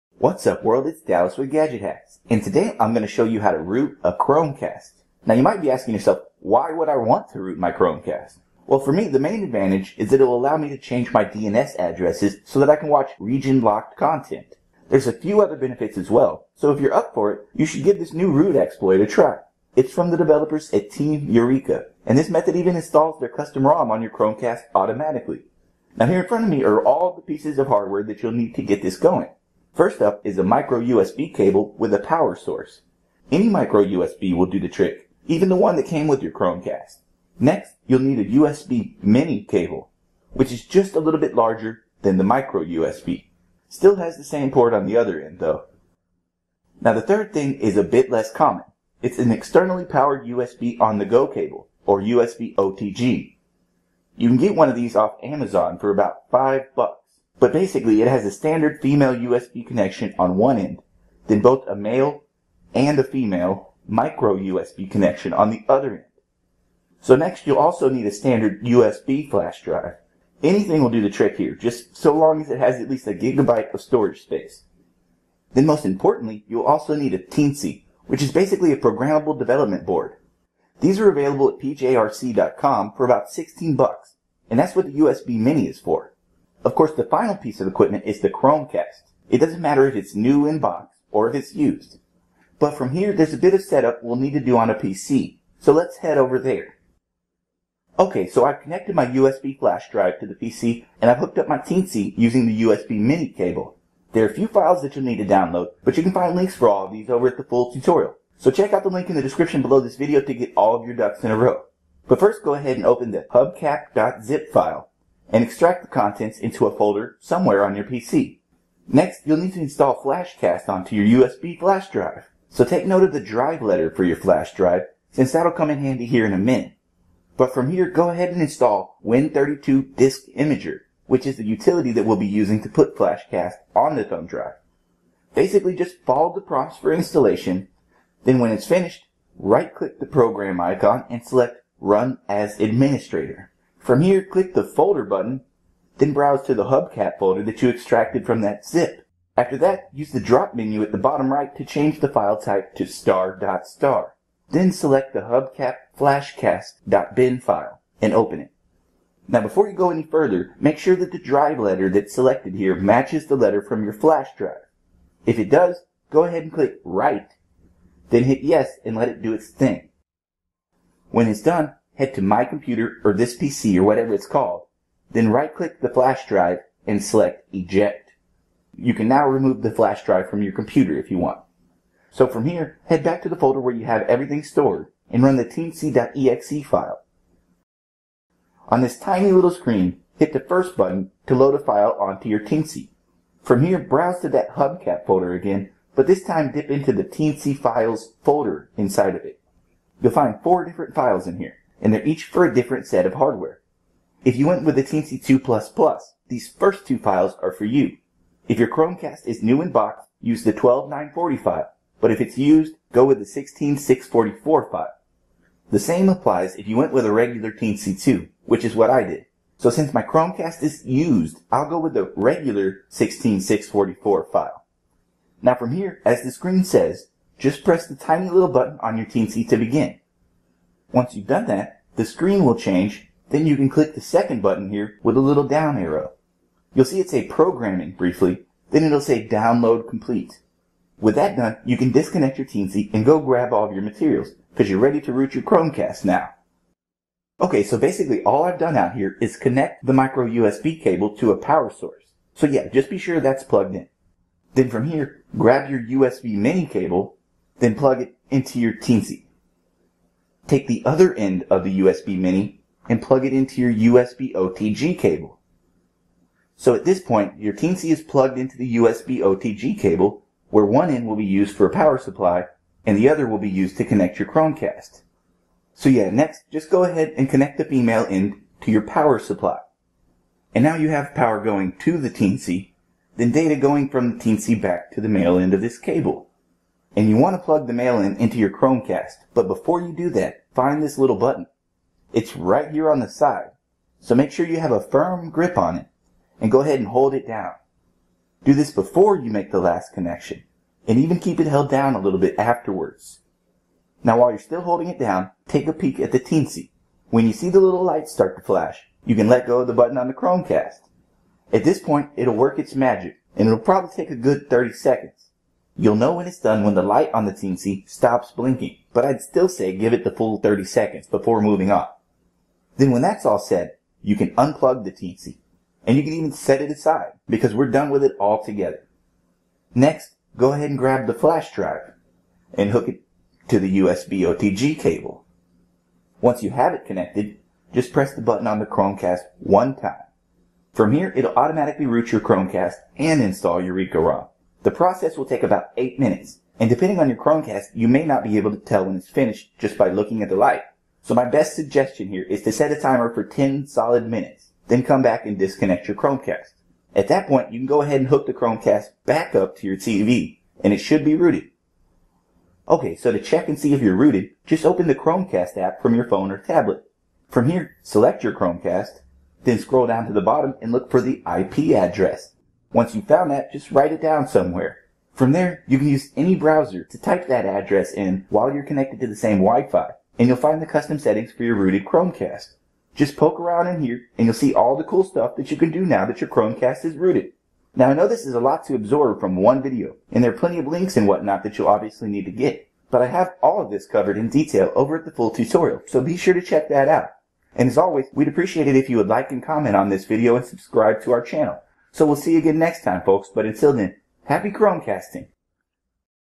Root Your Chromecast & Install a Custom ROM [How-To]
How to Root Your Chromecast

In this tutorial, I'll be showing you how to root ANY Chromecast, regardless of the firmware version.

Team Eureka just released a new exploit called "HubCap" that is capable of rooting even the latest version of Chromecast's firmware. With this method, you'll be able to root and install a custom ROM on your Chromecast all at once.

There are a few files that you'll need to download, and some hardware is required. Everything you need can be found at the full tutorial, though.